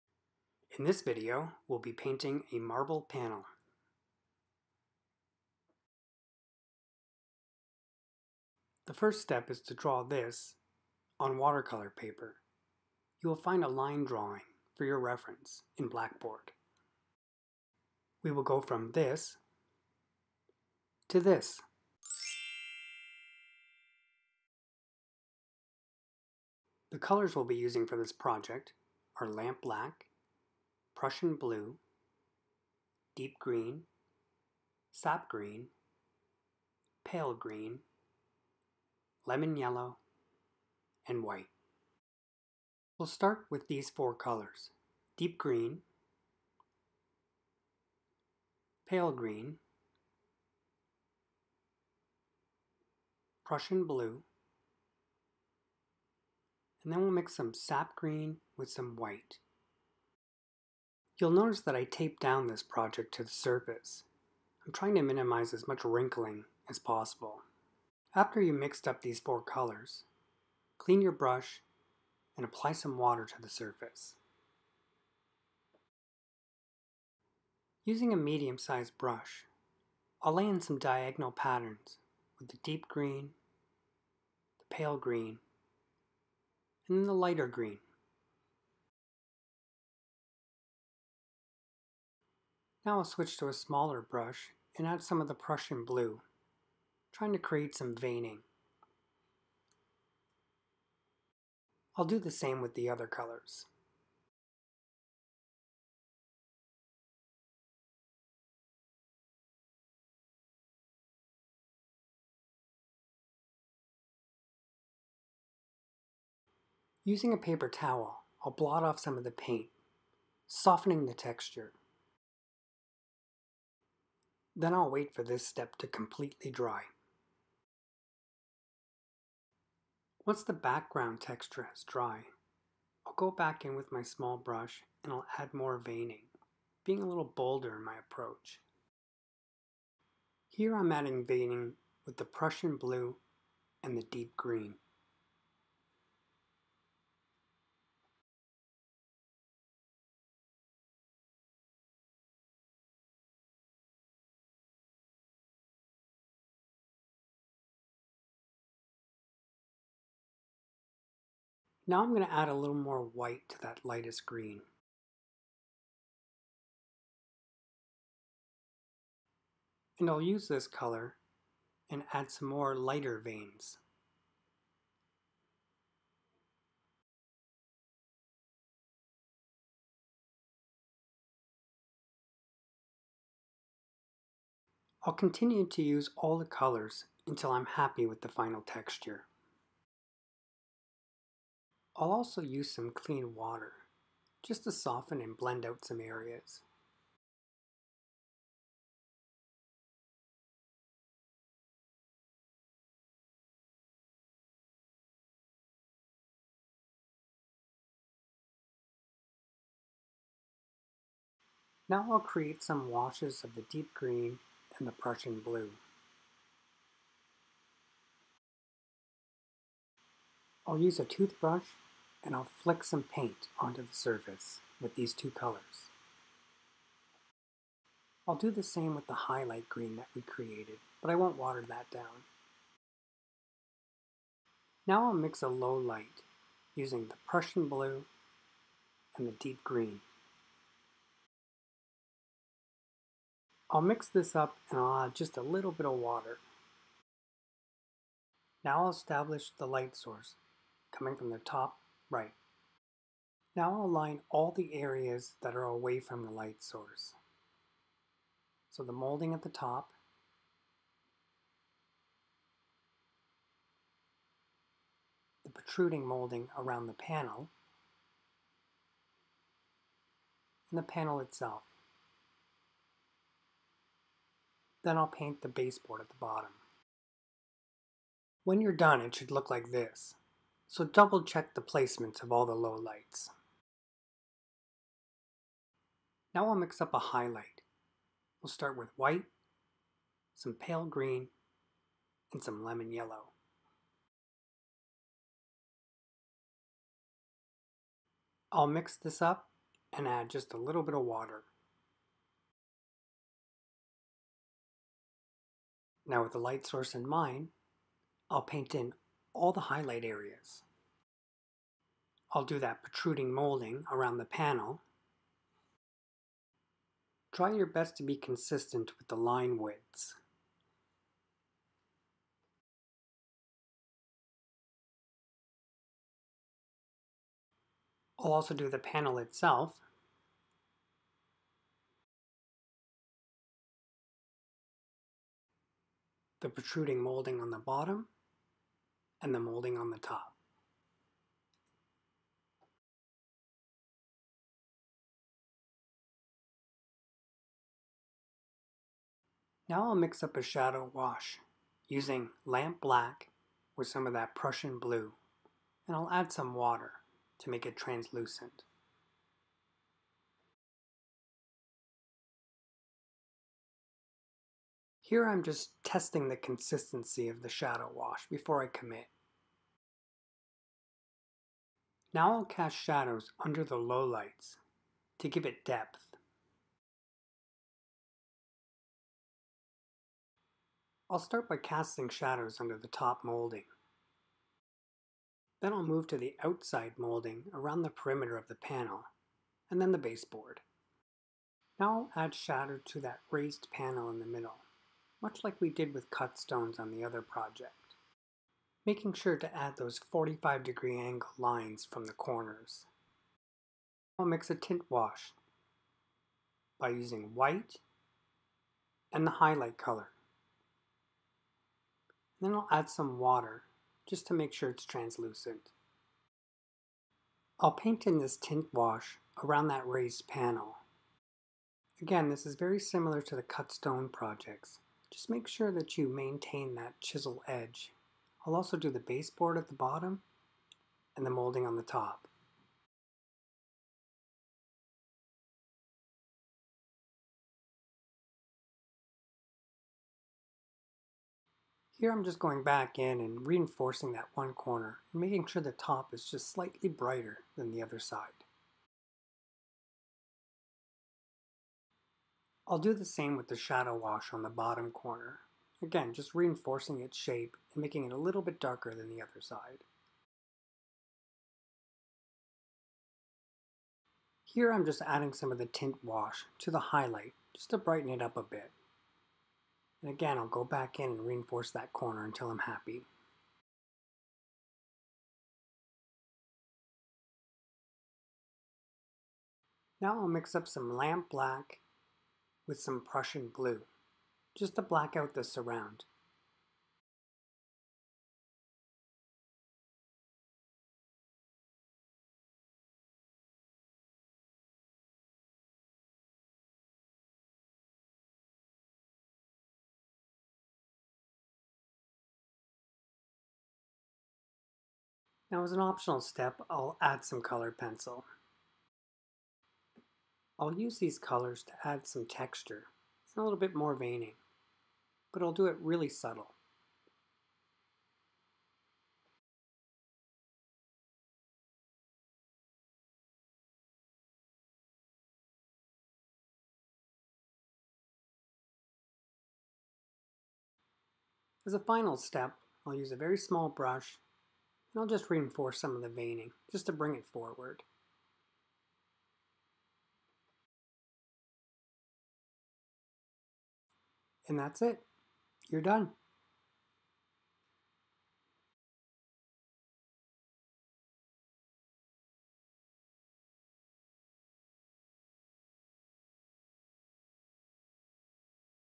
Scene Painting Elevations - Marble
In this video, you will learn how to create a marble texture with a raised panel and molding. You will mix a variety of colours and create a three-dimension illusion using a highlight, lowlight, tint wash and shadow wash with a light source coming from the top right.